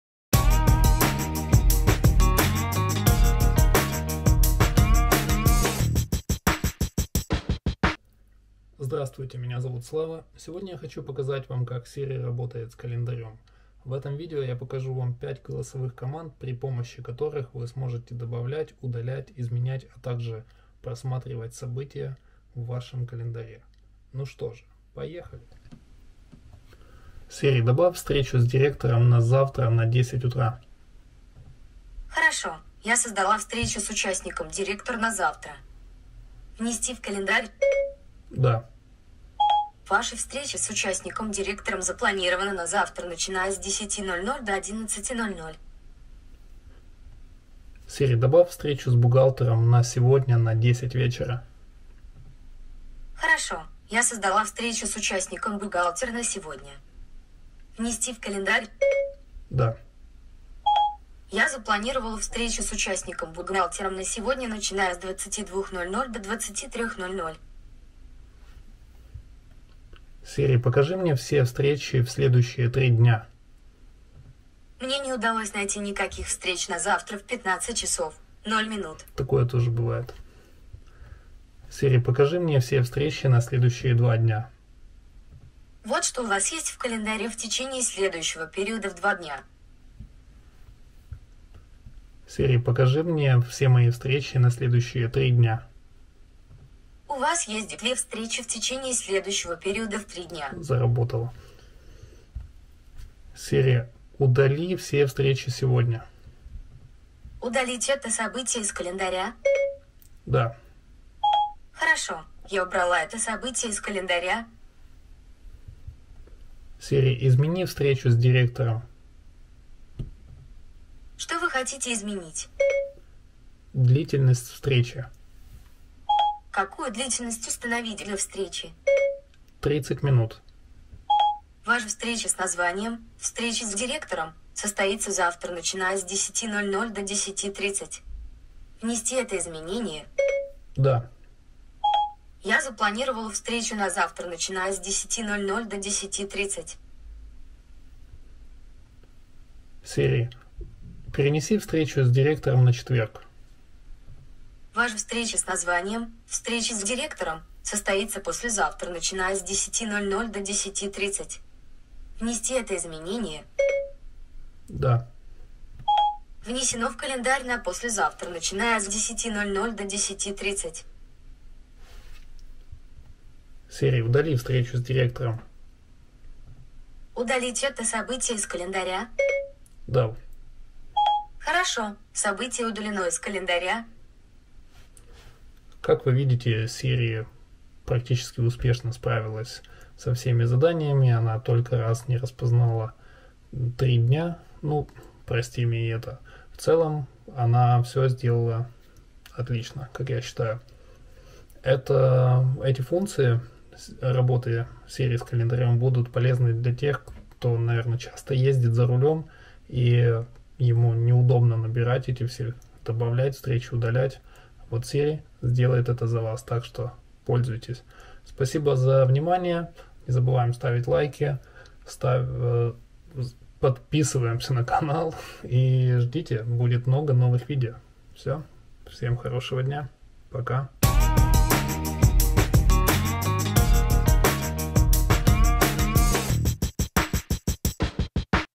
SIRI И КАЛЕНДАРЬ | iOS 14 | КОМАНДЫ КОТОРЫЕ ПОМОГУТ ВАМ ОРГАНИЗОВАТЬ ВАШ КАЛЕНДАРЬ
В этом видео я рaсказал о командах Siri которые помагают мне организовывать мой календарь и могут пригодиться кому-нибудь, кто не знаком с ними.

Продукты Apple
Сири на русском
Голосовой помошник Сири
Производительность с помошью Сири